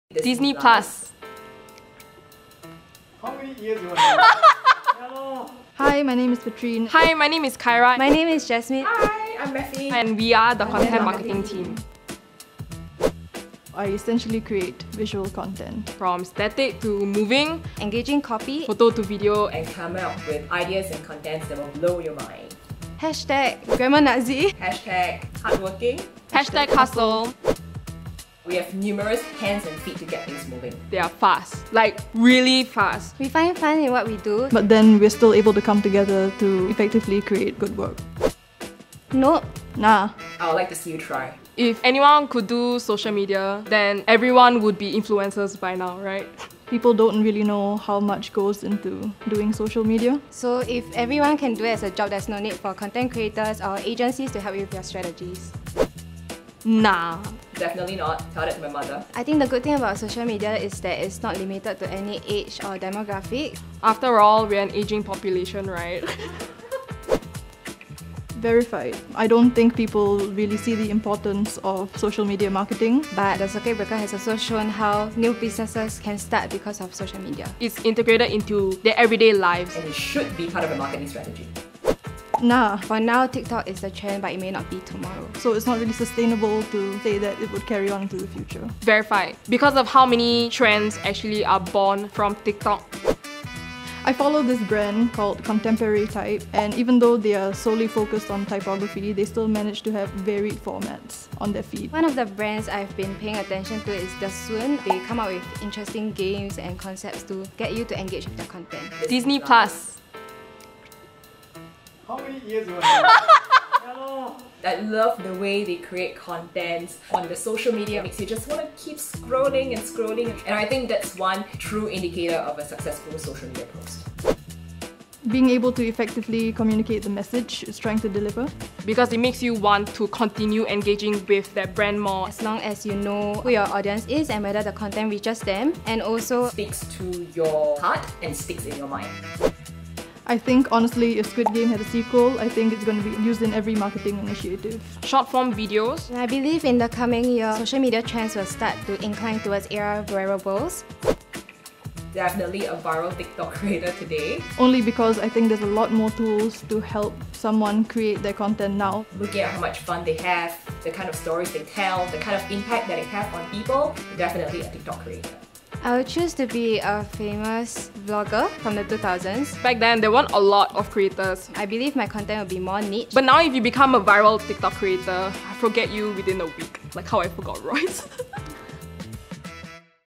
Fresh Look: Superminted's Content Marketing Team
Meet the agency's pop-culture gurus, trend connoisseurs and visual storytellers, the Content Marketing Team!

In 180 seconds ⏰ find out what drives these zesty content creators and their hacks on social media success for brands.